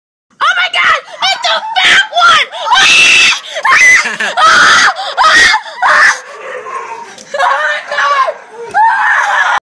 Dog vs raccoon
So yesterday morning there was a raccoon in my backyard and my dog was chasing it and  freaking out